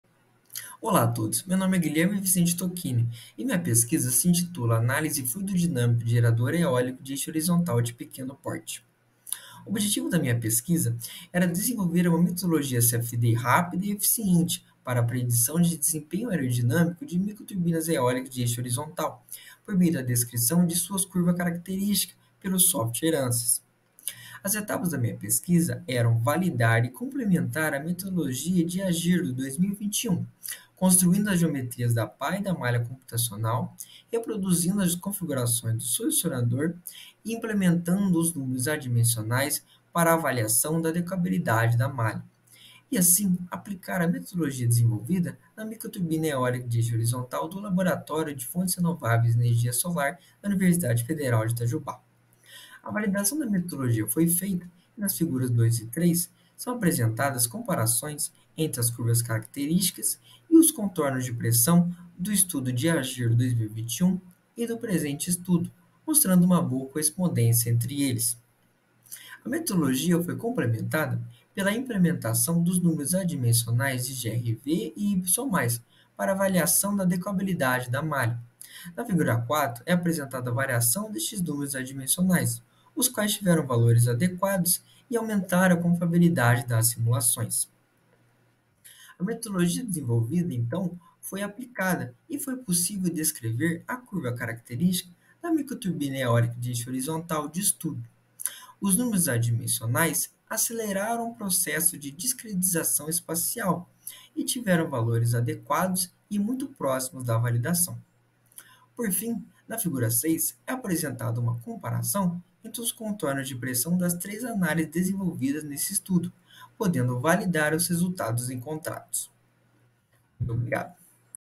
ANÁLISE FLUIDODINÂMICA DE GERADOR EÓLICO DE EIXO HORIZONTAL DE PEQUENO PORTE
Aluno: Guilherme Vicente Toquini

Orientador: Angie Lizeth Espinosa Sarmiento

Curso: Engenharia Mecânica

Programa: PIBIC CNPq